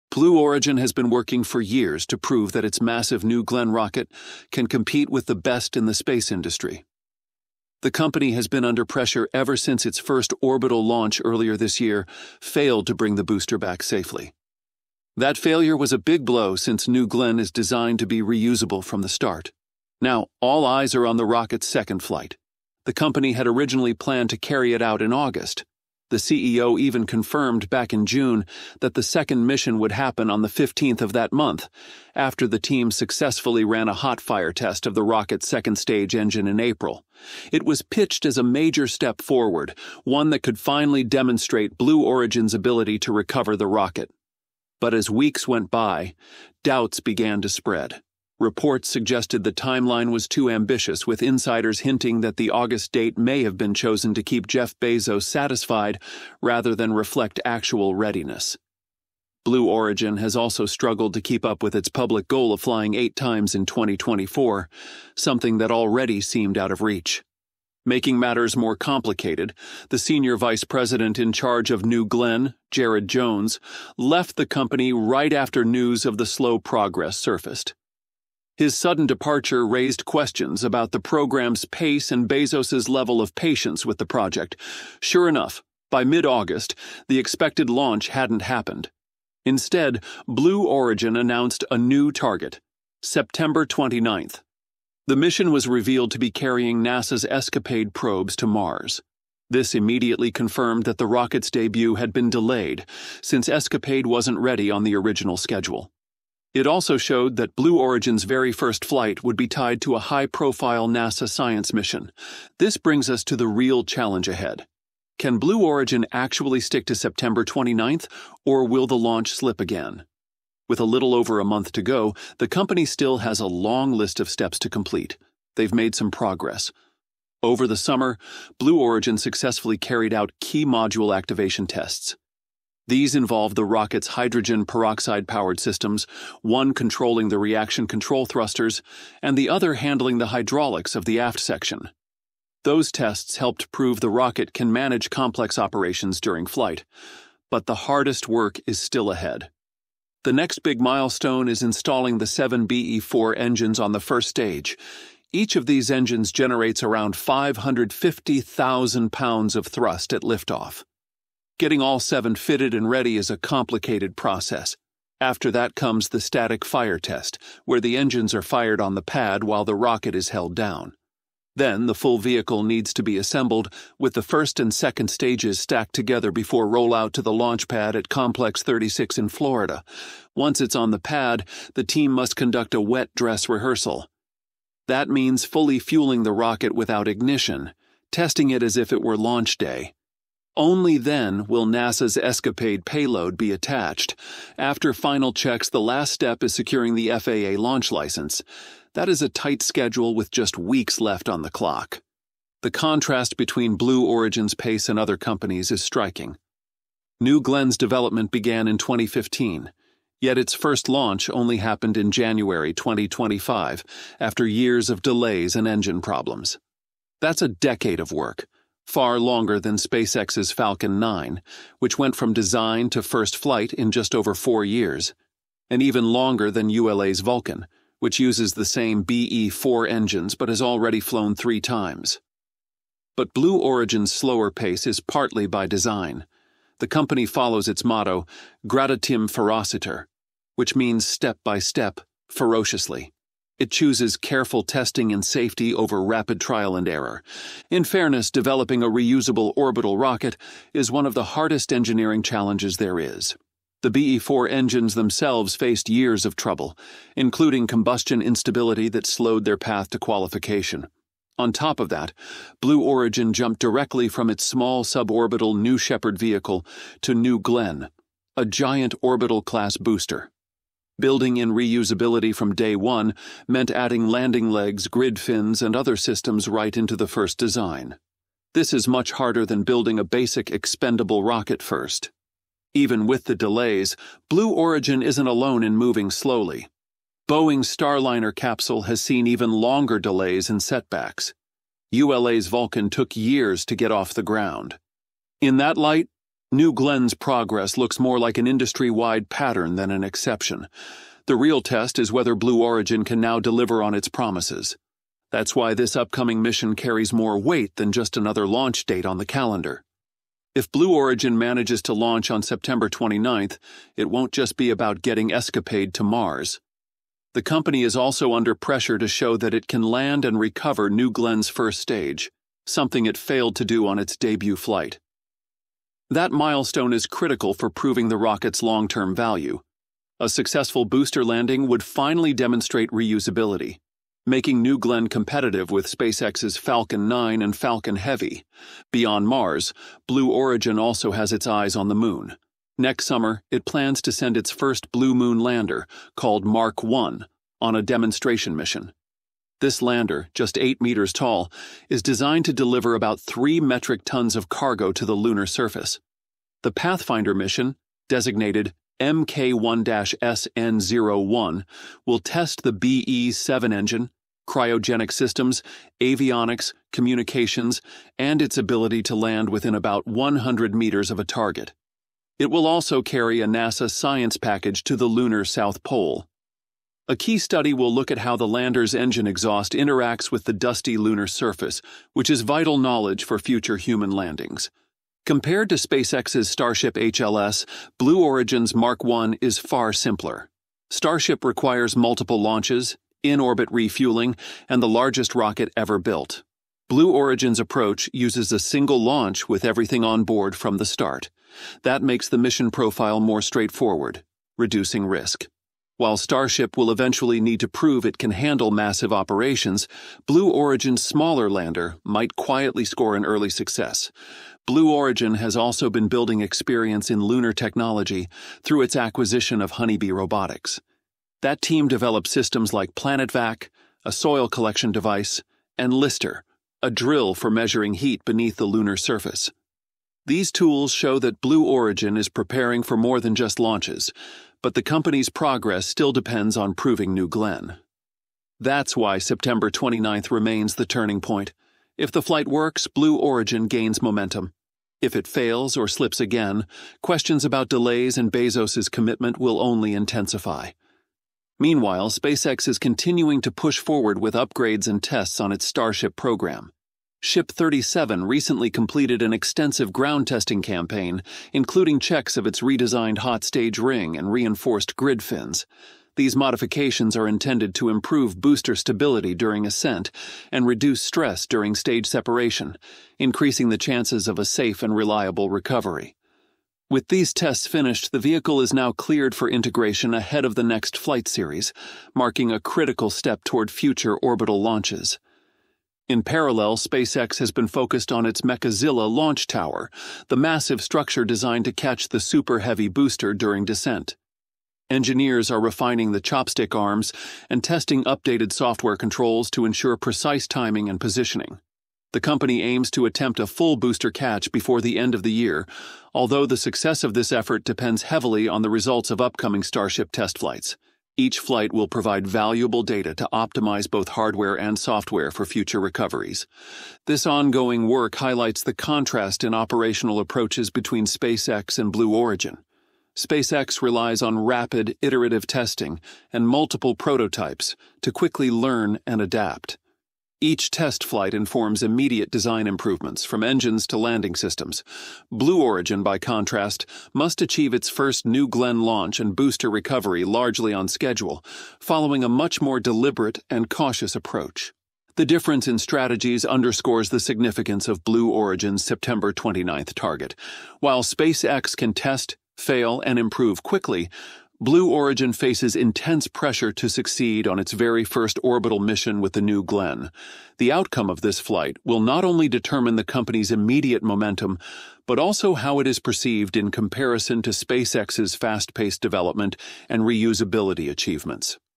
Blue Origin Failed To Launch Rocket Again While SpaceX is Breaking Record!
Blue Origin has been working for years to prove that its massive New Glenn rocket can compete with the best in the space industry. The company has been under pressure ever since its first orbital launch earlier this year failed to bring the booster back safely. That failure was a big blow, since New Glenn is designed to be reusable from the start. Now, all eyes are on the rocket’s second flight.
The company had originally planned to carry it out in August. The CEO even confirmed back in June that the second mission would happen on the 15th of that month, after the team successfully ran a hot-fire test of the rocket’s second stage engine in April. It was pitched as a major step forward, one that could finally demonstrate Blue Origin’s ability to recover the rocket. But as weeks went by, doubts began to spread. Reports suggested the timeline was too ambitious, with insiders hinting that the August date may have been chosen to keep Jeff Bezos satisfied rather than reflect actual readiness. Blue Origin has also struggled to keep up with its public goal of flying eight times in 2024, something that already seemed out of reach.
Making matters more complicated, the senior vice president in charge of New Glenn, Jared Jones, left the company right after news of the slow progress surfaced. His sudden departure raised questions about the program’s pace and Bezos’s level of patience with the project. Sure enough, by mid-August, the expected launch hadn’t happened. Instead, Blue Origin announced a new target: September 29th. The  mission was revealed to be carrying NASA’s EscaPADE probes to Mars. This immediately confirmed that the rocket’s debut had been delayed, since EscaPADE wasn’t ready on the original schedule. It also showed that Blue Origin’s very first flight would be tied to a high-profile NASA science mission.